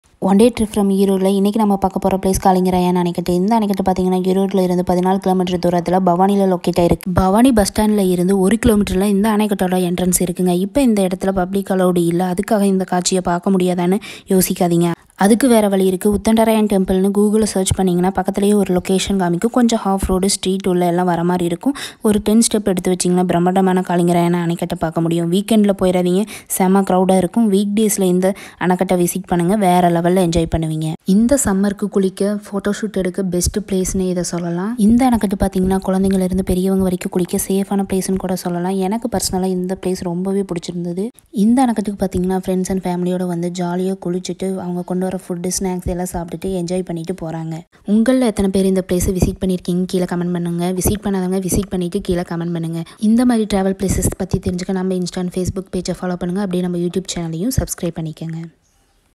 ஈரோட்டின் பிரம்மாண்ட அணை 😍 Kalingarayan Anaicut🔥
The anicut was constructed in the 13th century by the Kongu Tamil Chieftain Mamannar Kalingarayan Gounder and his birth name is Lingaiya Gounder. This irrigation project involved the construction of a dam and barrage to divert the water to Kalingarayan Canal. It is one of the ancient River linking project, which established the link between Bhavani river and Noyyal river. The Kalingarayan Anicut was built in 1283.

I put a lot of effort into making this video so